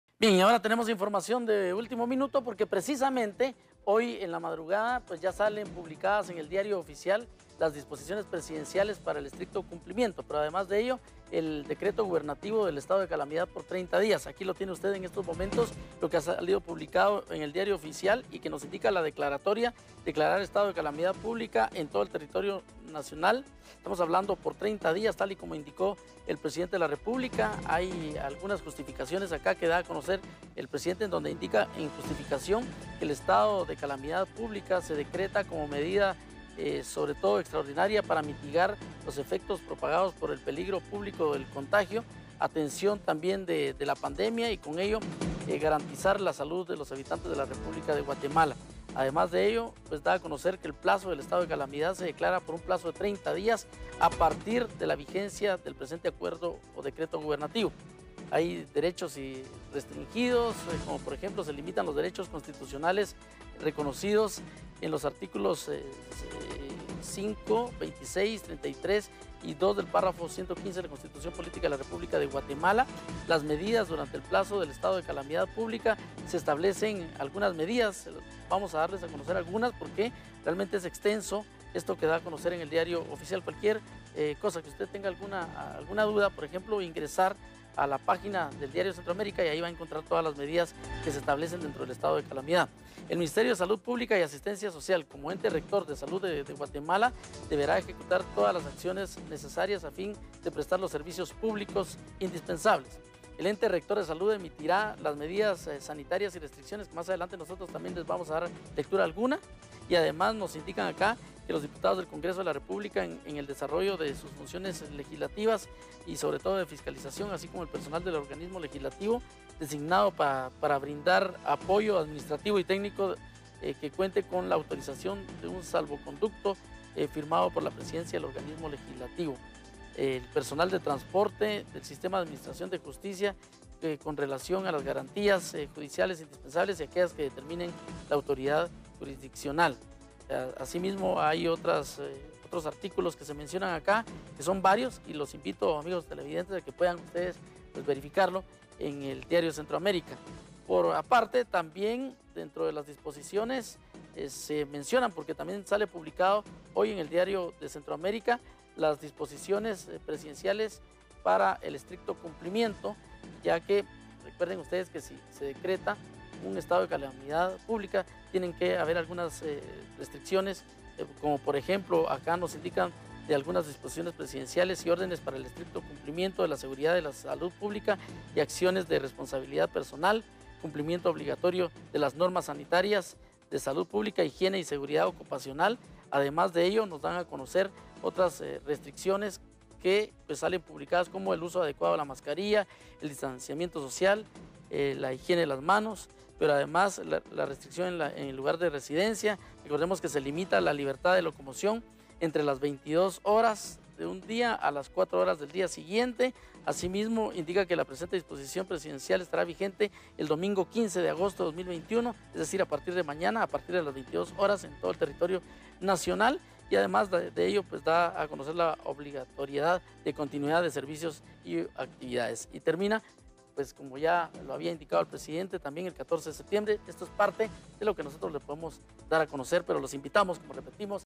Gobierno impone "estado de calamidad pública" por 30 días
Poder Ejecutivo decretó "estado de calamidad pública" por 30 días, aseguran que esta medida extraordinaria ayudaría a mitigar contagios de casos de coronavirus. Se restringen derechos constitucionales.